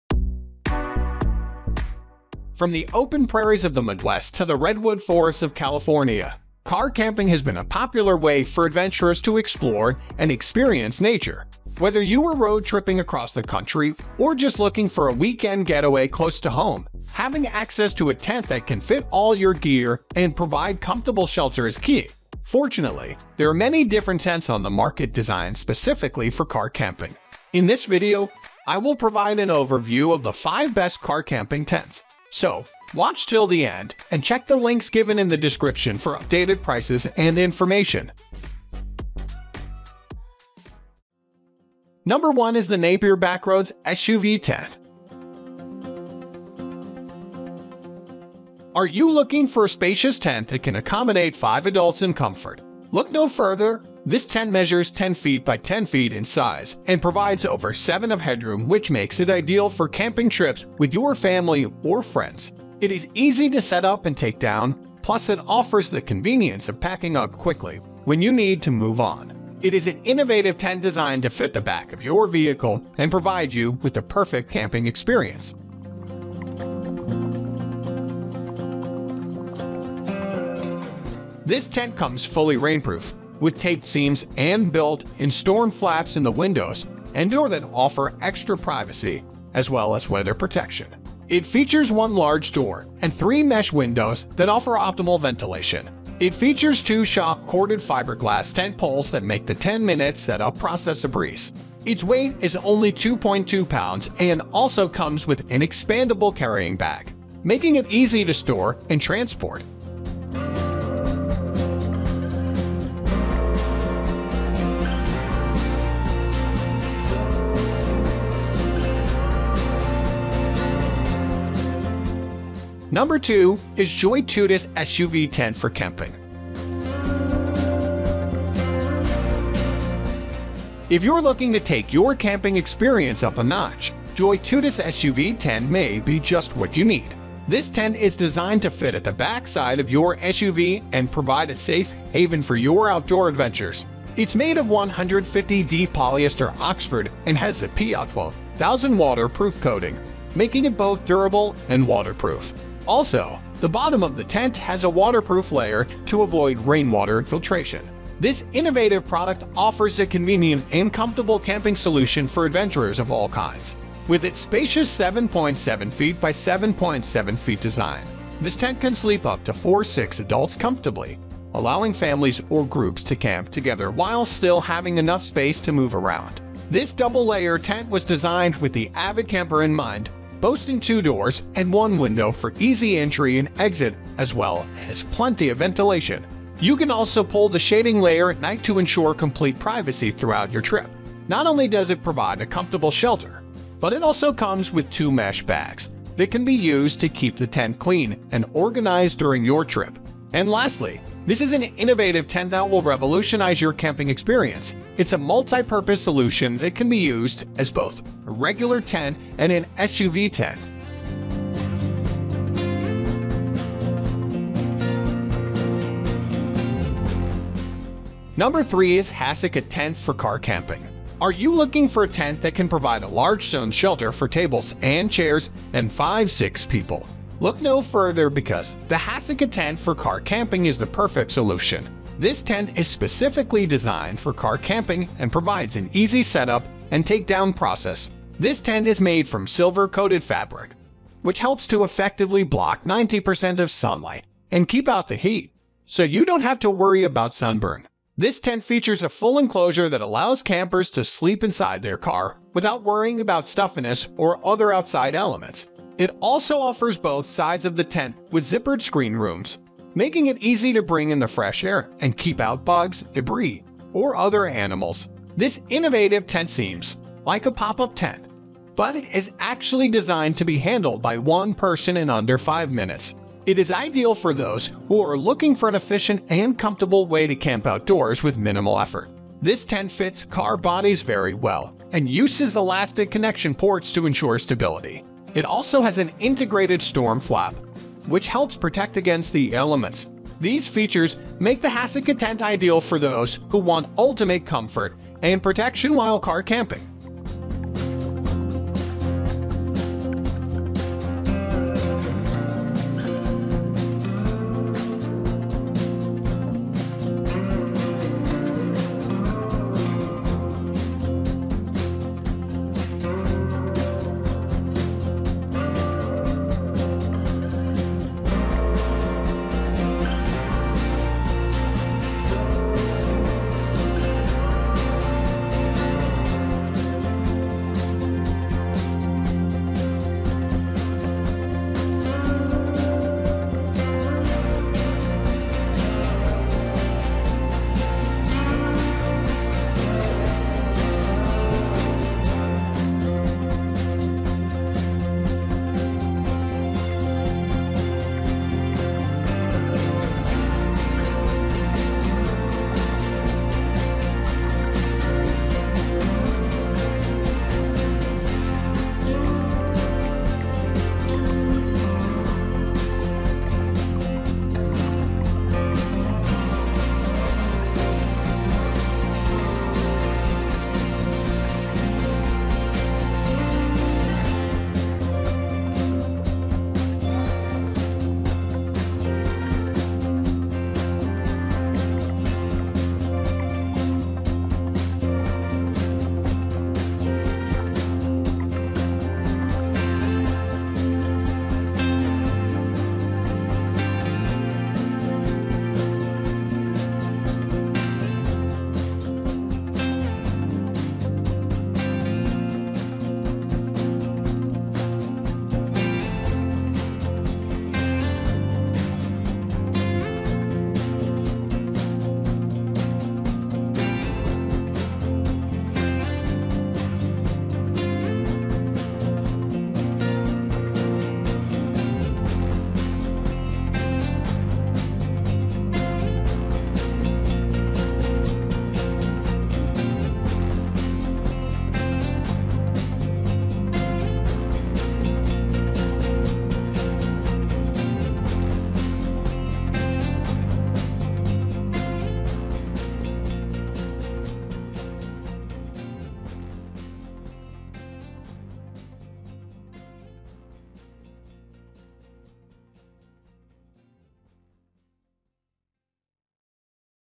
Top 5 Best Car Camping Tents in 2023 | Travel and Explore with Comfort
Easily Carry Exploration: The Best Car Camping Tents for You
From the open prairies of the Midwest to the redwood forests of California, car camping has been a popular way for adventurers to explore and experience nature. Whether you are road-tripping across the country or just looking for a weekend getaway close to home, having access to a tent that can fit all your gear and provide comfortable shelter is key. Fortunately, there are many different tents on the market designed specifically for car camping. In this video, I will provide an overview of the 5 best car camping tents.

00:00 Intro

00:41 Napier Backroadz SUV Tent

02:02 JoyTutus SUV Tent for Camping

03:38 Hasika Tent for Car Camping

04:57 KAMPKEEPER SUV Car Tent

06:50 Napier Backroadz SUV Tent

About These Products:

Napier Backroadz SUV Tent:
Spacious tent measuring 10 feet by 10 feet, accommodating 5 adults comfortably.
Provides over 7 feet of headroom for a comfortable camping experience.
Easy to set up and take down, with a quick packing process.
Fully rainproof with taped seams and built-in storm flaps for weather protection.
Features 1 large door and 3 mesh windows for optimal ventilation.
Comes with two shock-corded fiberglass tent poles for easy setup.
Lightweight at 2.2 pounds and includes an expandable carrying bag for easy transport.

JoyTutus SUV Tent for Camping:
Designed to fit at the back side of your SUV, providing a safe haven for camping adventures.
Made of durable 150D polyester oxford with PU2000 water-proof coating.
Spacious 7.7 feet by 7.7 feet design, accommodating 4-6 adults comfortably.
Features two doors and one window for easy entry, exit, and ventilation.
Double-layer tent for added comfort and privacy.
Comes with two mesh bags for keeping the tent clean and organized during the trip.
Multi-purpose tent that can be used as a regular tent or an SUV tent.

Hasika Tent for Car Camping:
Specifically designed for car camping with a large sun shelter for tables, chairs, and 5-6 people.
Made from silver-coated fabric, blocking 90% of sunlight and heat.
Full enclosure allows campers to sleep inside their car comfortably.
Both sides of the tent have zippered screen rooms for ventilation and insect protection.
Easy setup by one person in under five minutes.
Elastic connection ports ensure stability and a secure fit on car bodies.
Integrated storm flap for additional weather protection.

      leave us a message so we can